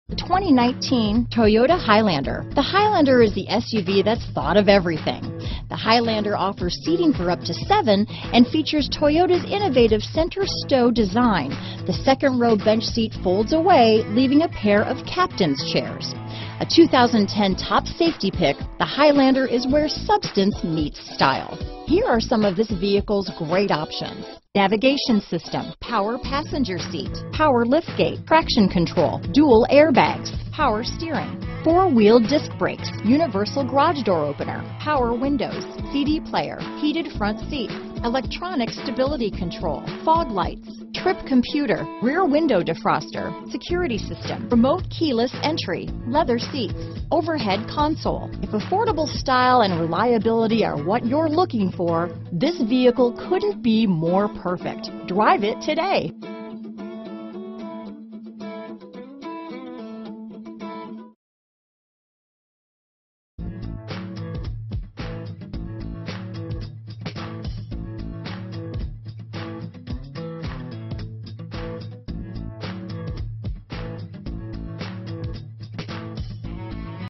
2019 Toyota Highlander Jacksonville, Arlington, Orange Park, Nocatee, Palencia, FL KS308900
Shoreline Blue Pearl New 2019 Toyota Highlander available in Jacksonville, Florida at Coggin Toyota (Ave). Servicing the Arlington, Orange Park, Nocatee, Palencia, FL area.